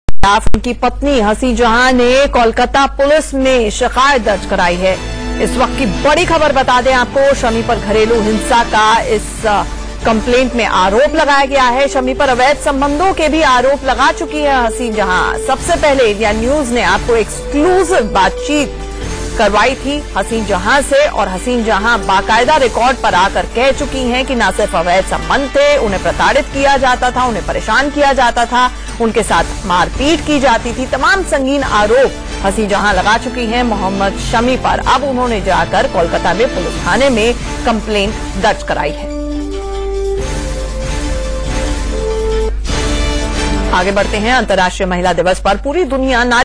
क्रिकेटर मोहम्मद शमी के खिलाफ उनकी पत्नी हसीन ने कोलकाता थाने में केस दर्ज कराया
क्रिकेटर मोहम्मद शमी के खिलाफ उनकी पत्नी हसीन जहां ने कोलकाता पुलिस में शिकायत दर्ज कराई है । शमी पर घरेलू हिंसा का आरोप लगाया गया है । शमी पर अवैध संबंधों का भी आरोप लगा चुकी हैं ।